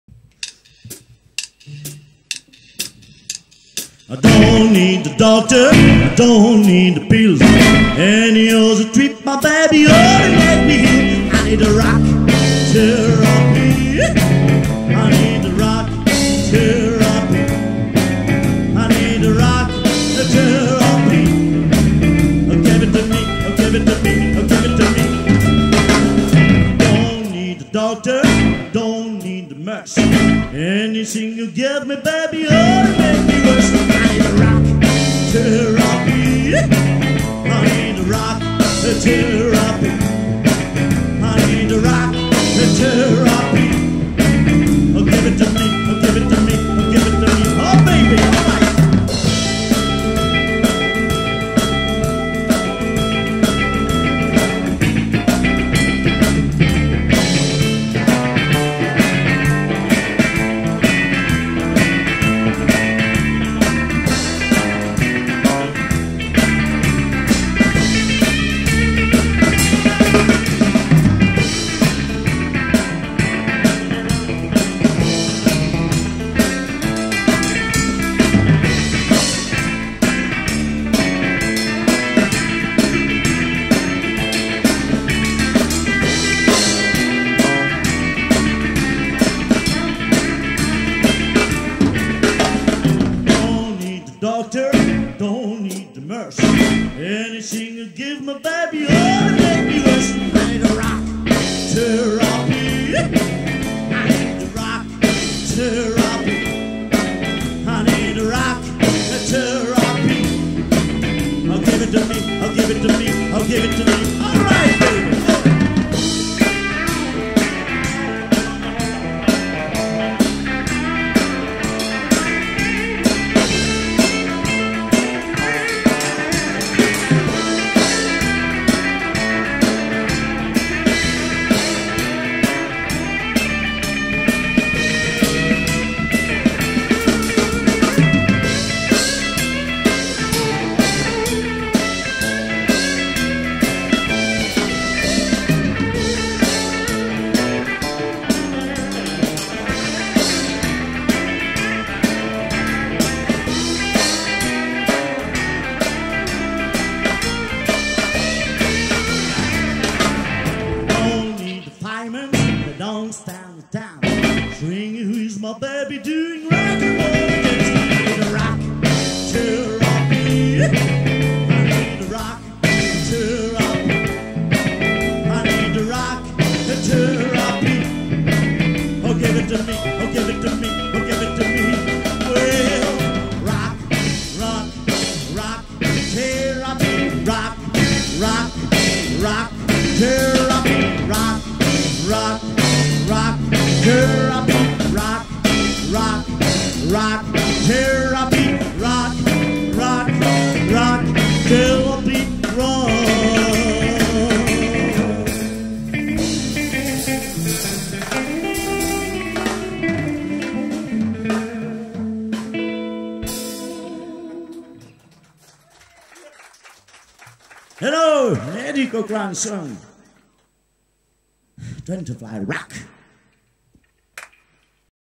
EVIL BUMBLEBEE 09
Captation du passage du groupe en direct sur le plateau de le 2 novembre 2014